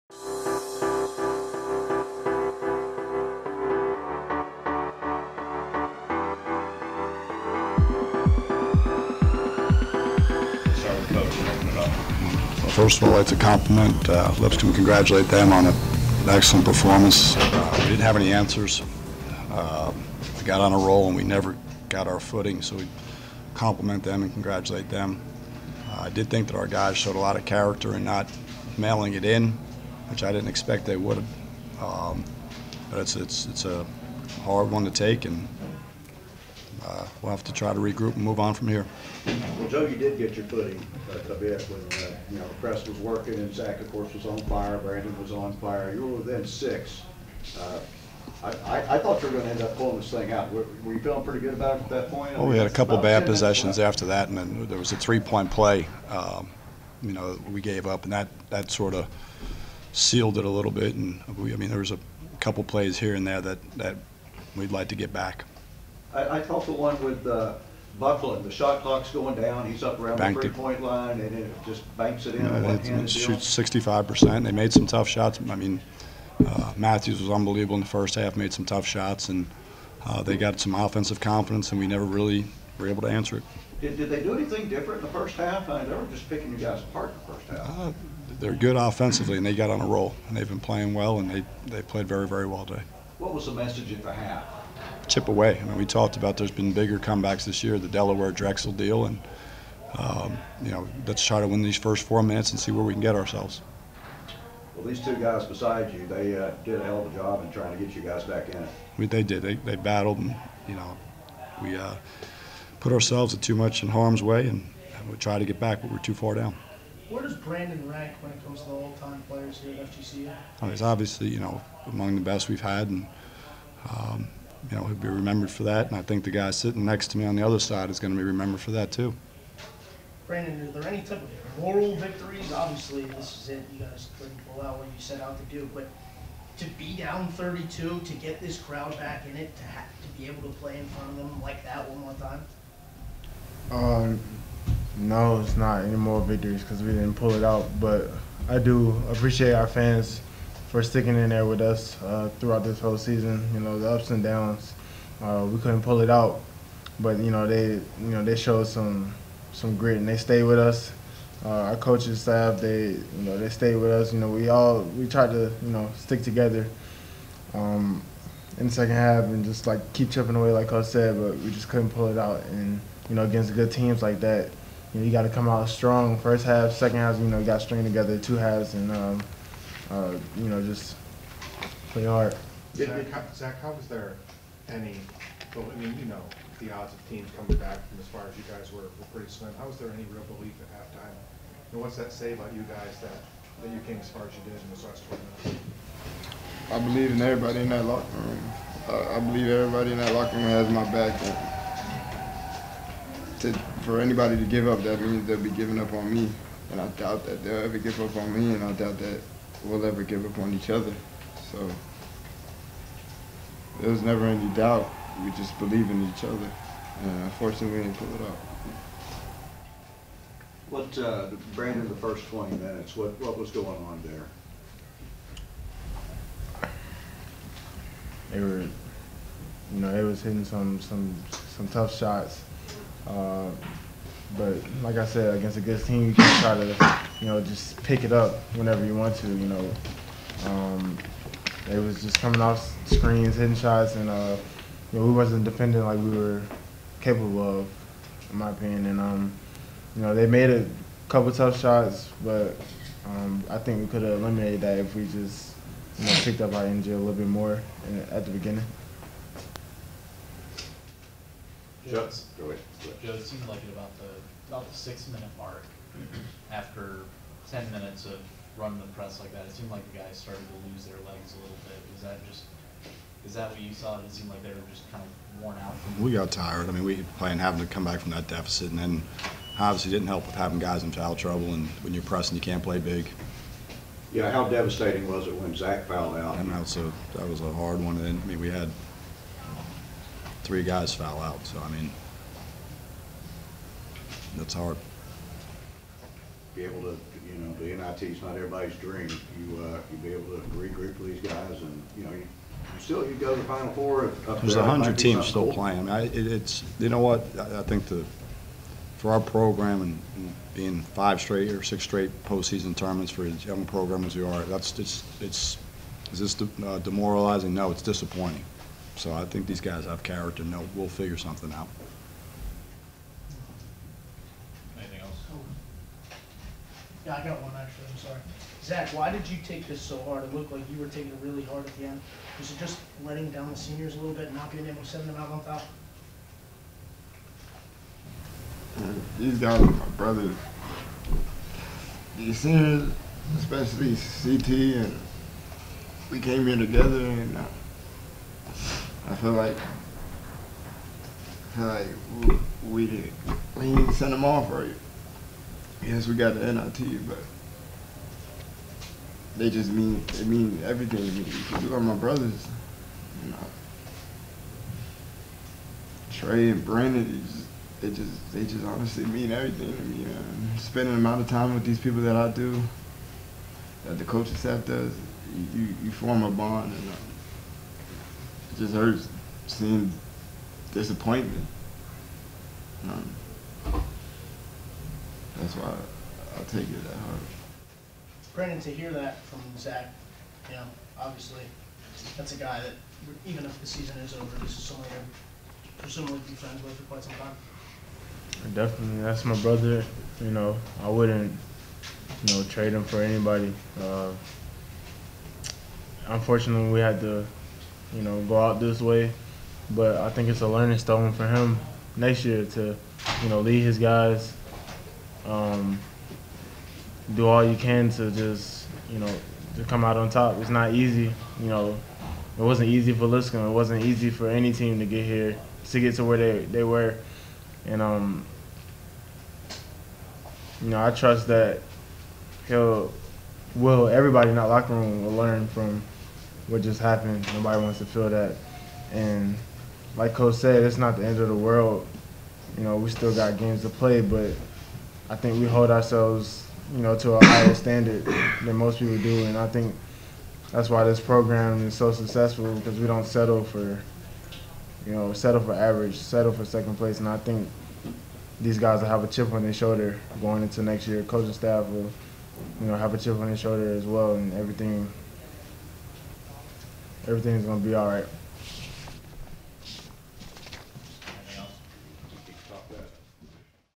presser - ASUN Final - Lipscomb
The mens basketball team fell to Lipscomb today, 96-108. The team talks about what Lipscomb did right in the first half, this afternoon's disappointments and moving on to the NIT.